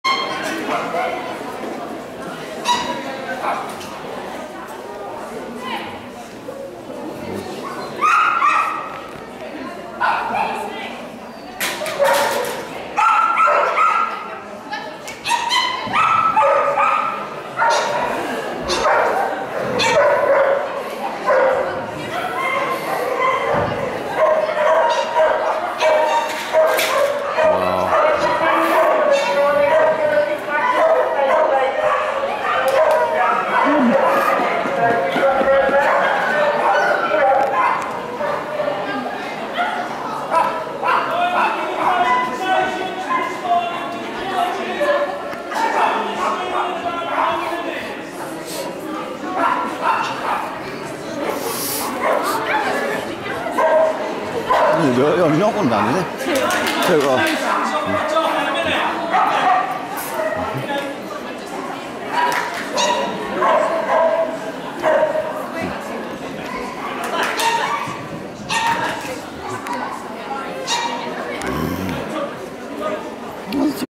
Marleys first agility show: Class 3 Power and speed. 11/12/11
Tailwaggers winter agility series 11th December 2011.
Complete the jumping course with no faults then progress to the contact equipment to knock 5 seconds off for each contact hit.
Unfortunately knocked the first pole, my fault, followed the rest of the course beautifully, hit another pole out of the tunnel but did very well. 10 faults, 7th place.